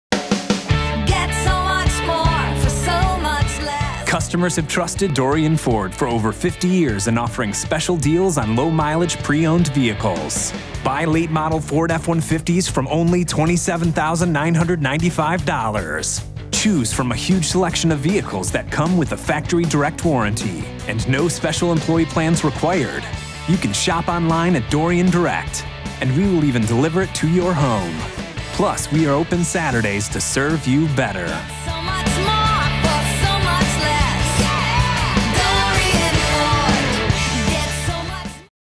Ford F-150 from $27,995 only at Dorian Ford
Bring home a quality, pre-owned, low mileage Ford F-150 from only $27,995 at Dorian Ford!

Customers have trusted Dorian Ford for over 50 years in offering special deals on low mileage, pre-owned vehicles. Choose from a huge selection of vehicles that come with a factory direct warranty and no special employee plans required. Shop online today at DorianDirect.com and we can bring it to you. Or stop in, even on Saturdays!